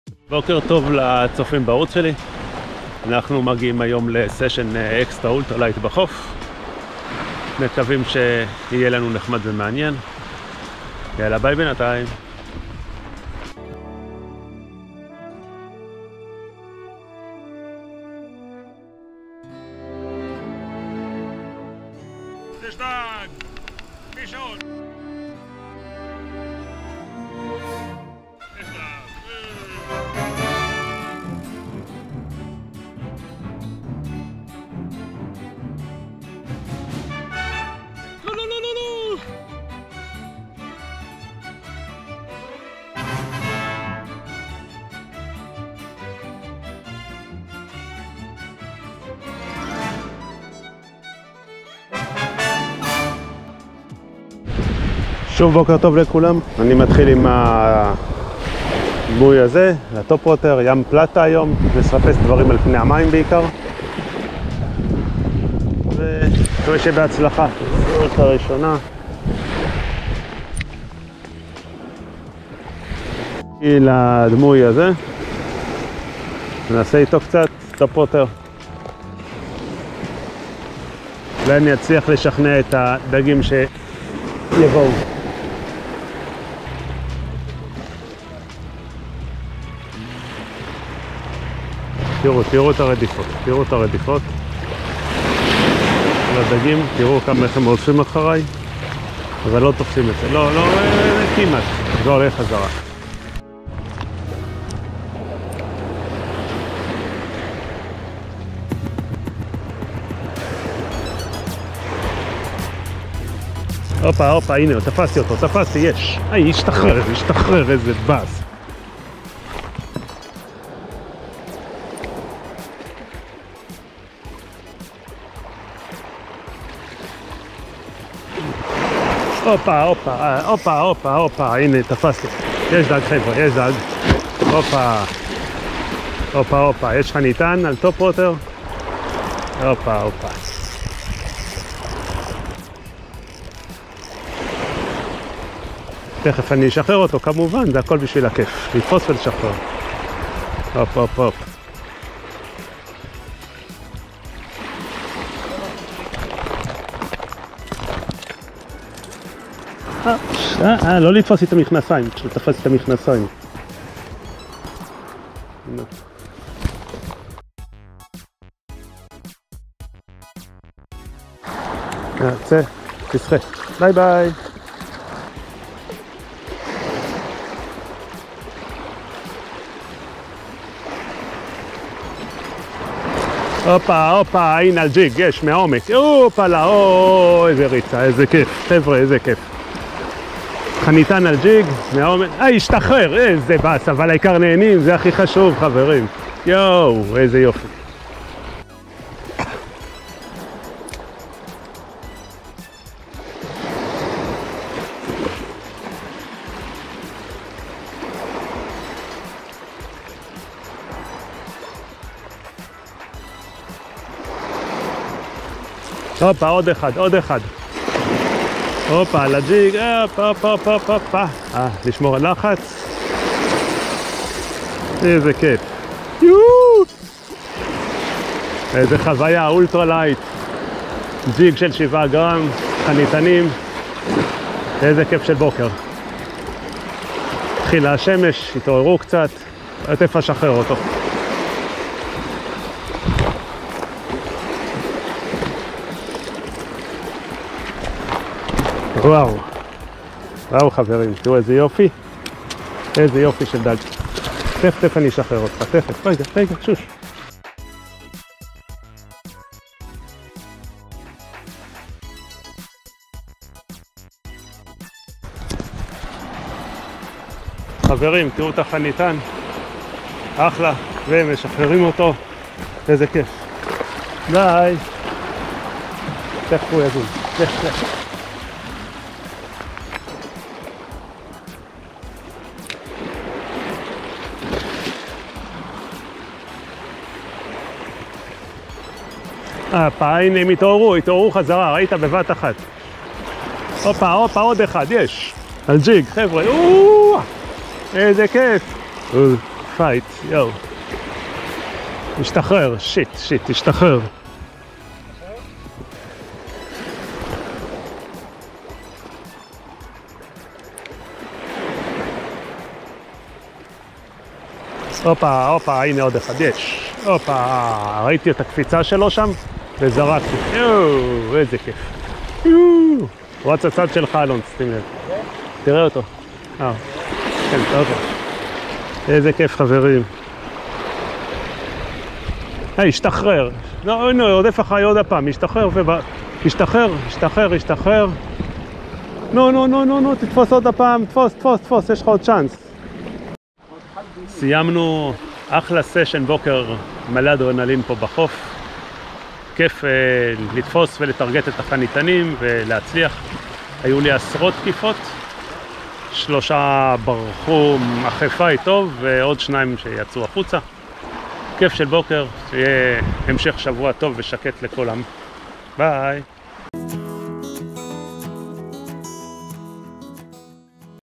בוקר מלא אקשן עם החניתנים 🔥 בז'רז'ור אולטרה לייט 🐟 A morning full of action in UL fishing HE/EN Subs
הפעם יצאנו כל הצוות לסשן חוף לחיפוש של החניתנים.
אני עם סט האולטרה לייט שלי ומתחיל עם טופווטרים שונים.
ככל שעולה השמש, מתחילים לראות את החניתנים במים שרודפים אחרי להקות דגי הפתיון ואני מתחיל לטרגט אותם.
יש תקיפות על הדמוי, אבל ללא נעילות או שבורחים באמצע ואז אני מצליח לנעול דג אחד ומתחיל פייט מהנה וחניתן ראשון לבוקר הזה מגיע לחוף 💪.
הוא מצליח להסתבך לי עם השיניים שלו והקרסים של הדמוי במכנסיים 🤷‍♂️ .

לאחר מכן, אני עובר לג'יג של 7 גרם (כדי להגיע רחוק יותר ולהביא משם את החניתנים).
עם הג'יג הקטן התחילה ממש חגיגה. עשרות של תקיפות, נעילות ובריחות .
הצלחתי בשלב מסויים לנעול עוד חניתן על הג'יג ולהביא אותו בבטחה לחוף.
כיף של בוקר מלא אקשן במים🔥 וזה העיקר, להנות מהדיג .

כל הדגים שוחררו בשלום👌.

מקווה שתהנו מהסרט של היום 🤝

הציוד שהשתמשתי בו:
רולר: Shimano Ultegra 2500 SHG
חוט: VARIVAS Max Power PE X8 #0.6
לידר: Berkley TRILENE 0.25mm
דמויים:
Jerry Blade Topwater Pencil 70mm
ג'יג 7 גרם צבע ורוד כסף